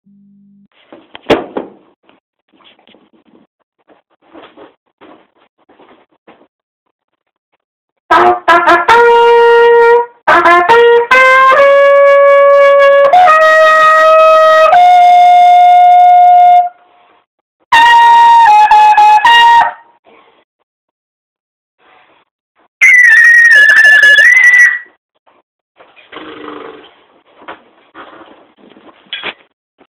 Trumpet Super C
Just playn around, nun serious, 4fun, I know my trumpet is Supa outta Tune! Lol